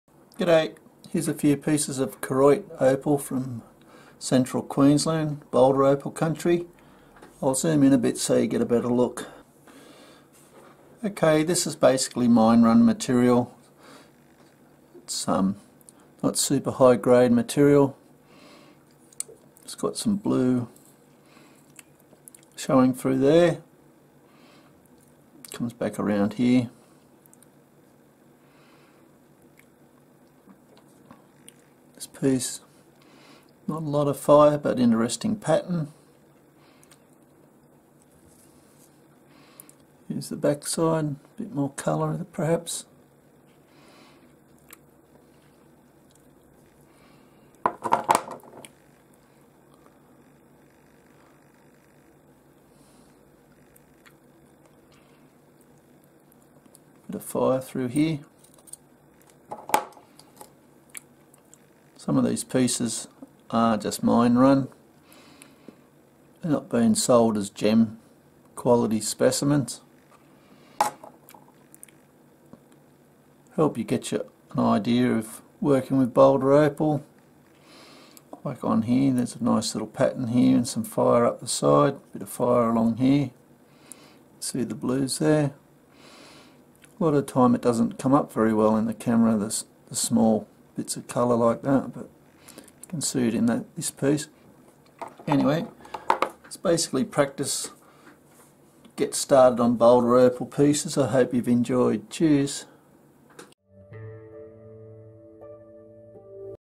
Koroit nut opal material from Queensland Australia - Ro1226
This Australian Boulder Opal material comes from Koroit in Outback Queensland. Interested? and type in the sku number in the search. It is at the end of the Description/ Title Also please note if the site says no Product found or similar it has more than likely been sold.